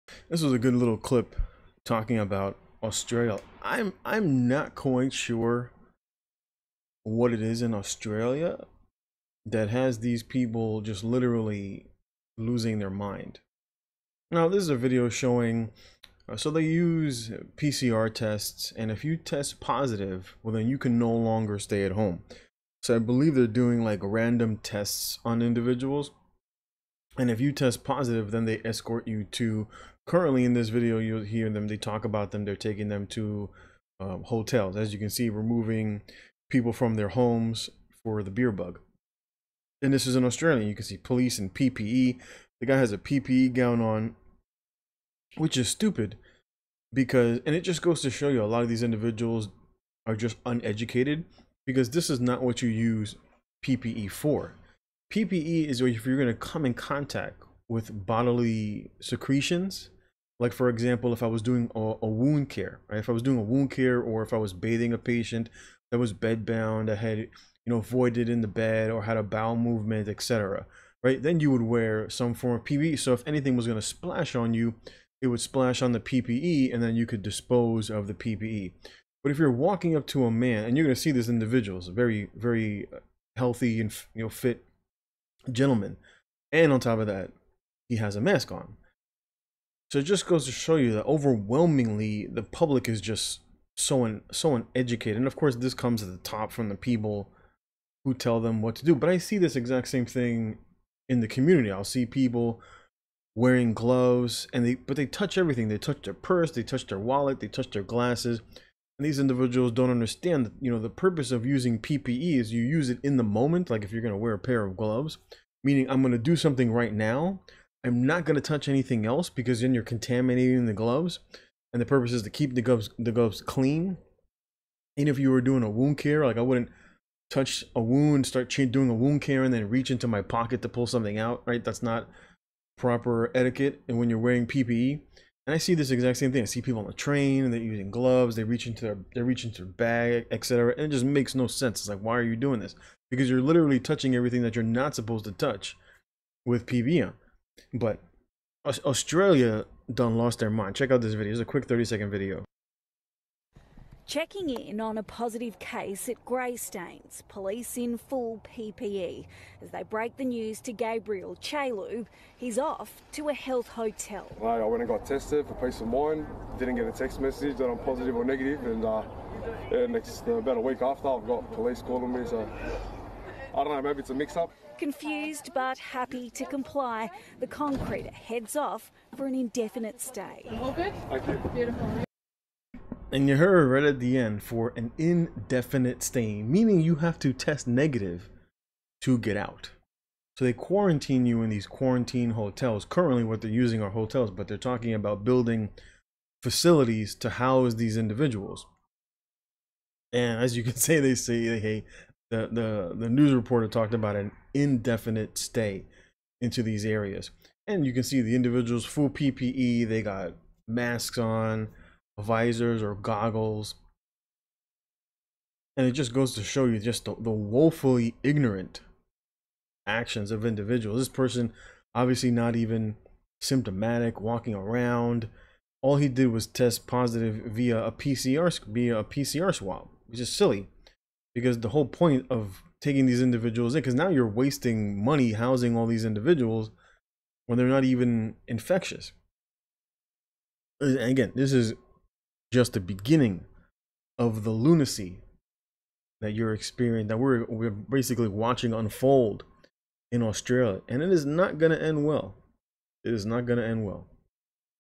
Australia Has Lost Their Mind, As Those Who Test Positive are Pulled From Their Homes
what will happened in Australia is left to be seen, but the ending will not be good.